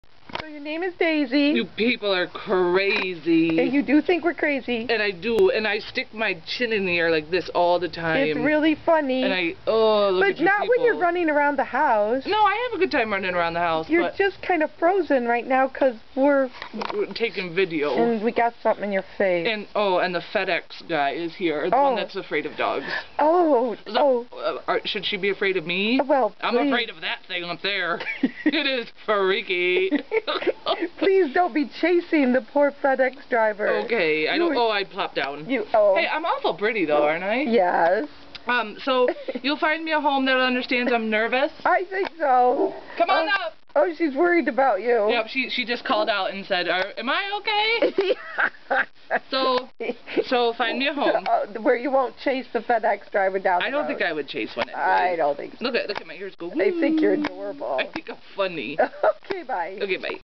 Video of adoptable pet named Daisy Barker
Daisy Barker is an adoptable pet with Joyful Rescues, Inc. in Cuba NY.